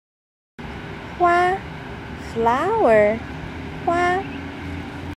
Lesson#18: Learn the Word Flower
This lesson teaches the word "flower" in Mandarin.